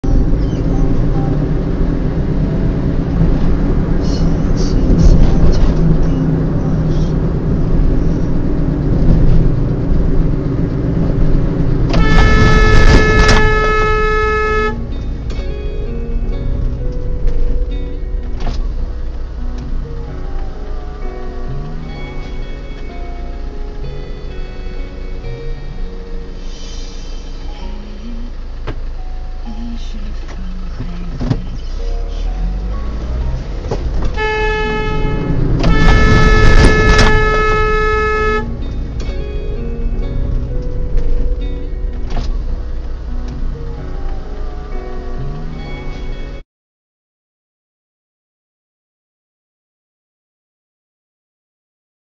14jun2020 cantonment road SMD4961A hyundai elantra fail to give way to vehicles from main road
14jun2020
cantonment road

SMD4961A hyundai elantra

fail to give way to vehicles from main road